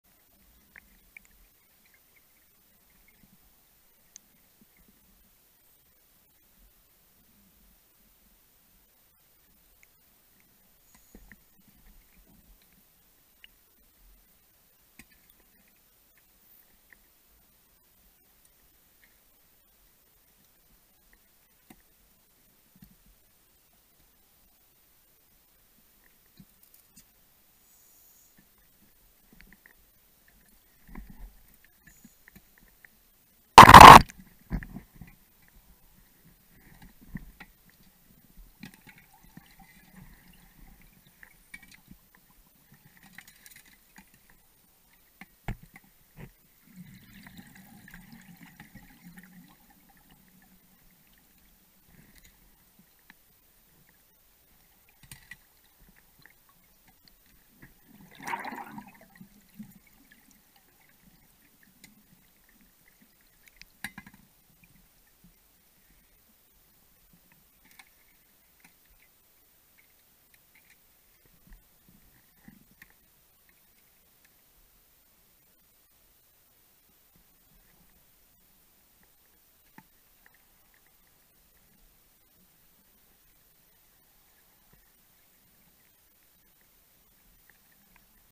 France - Source du Groin, clip through 2nd set of restrictions
Clip at the 2nd set of restrictions in sump 1 around 520m